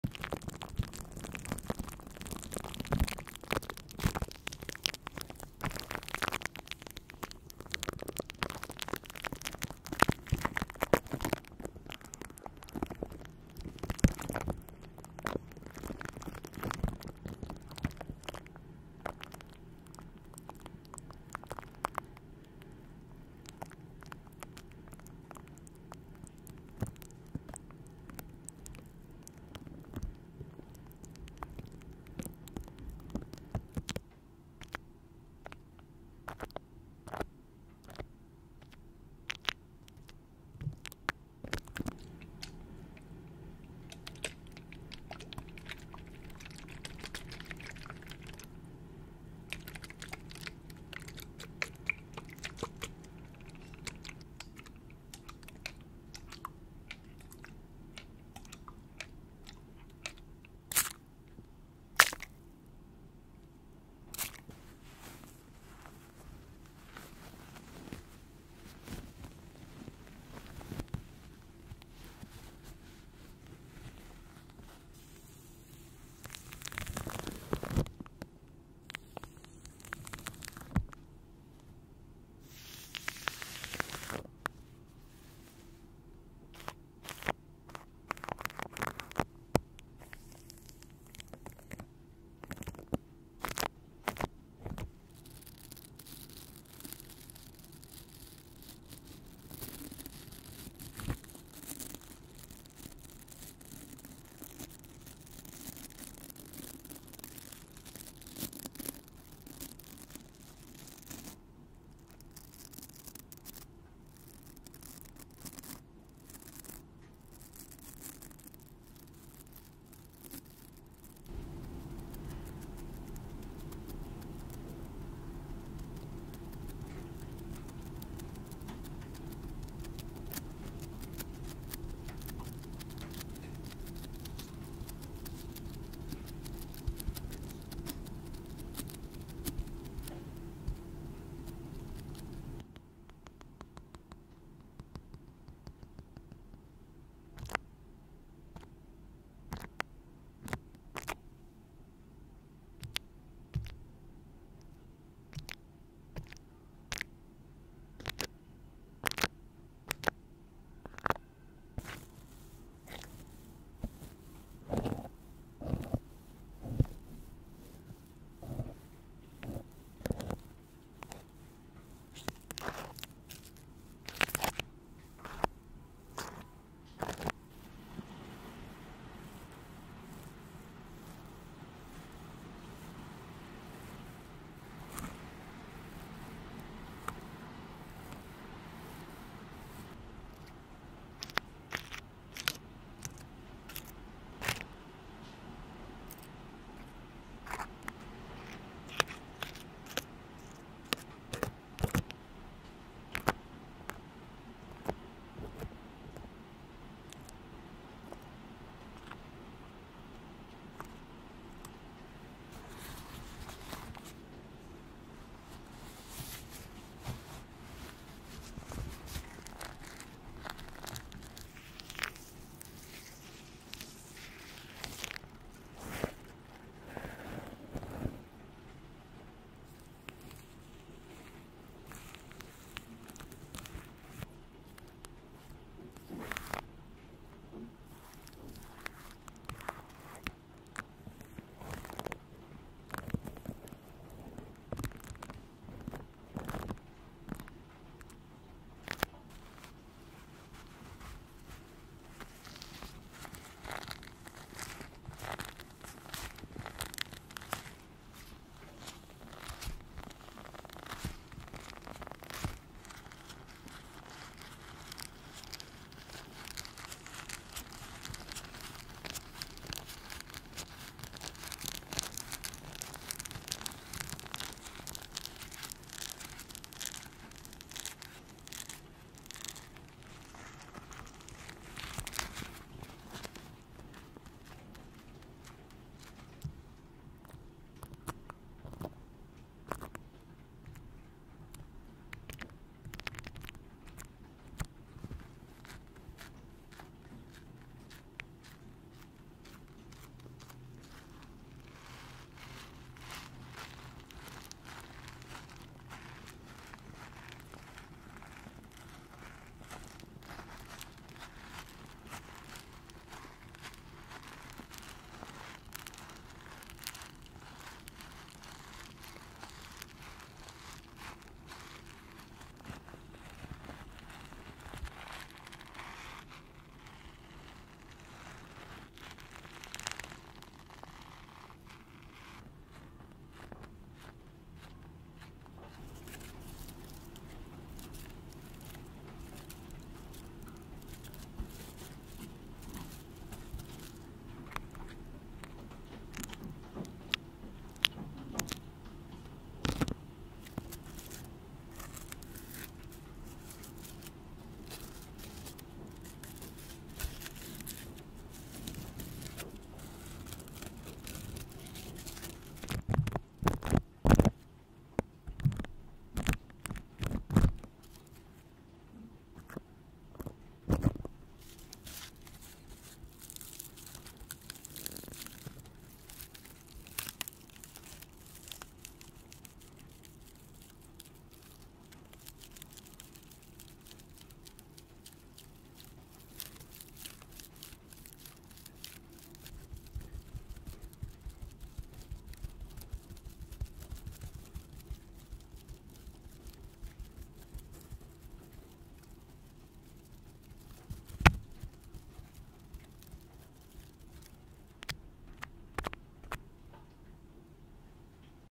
6 minutes essence of intense tingles with cleaning tools 【no talking】
I edited it from 30 minutes raw videos,I almost fell asleep when listening while editing it😂

Enjoy it.sleep well.see y'all!